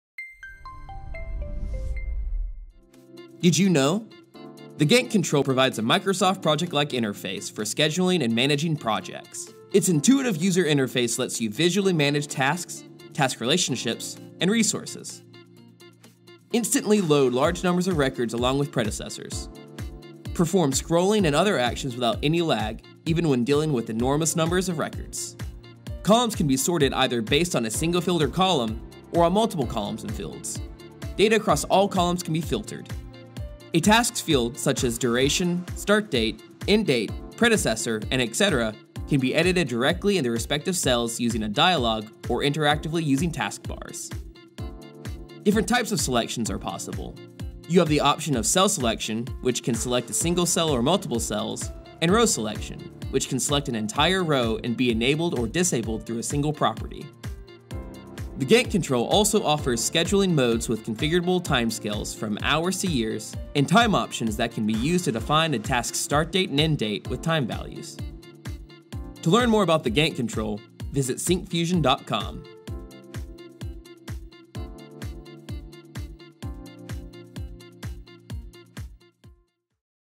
Gantt - Did You Know?
The Gantt control provides a Microsoft Project-like interface for scheduling and managing projects. Its intuitive user interface lets you visually manage tasks, task relationships, and resources.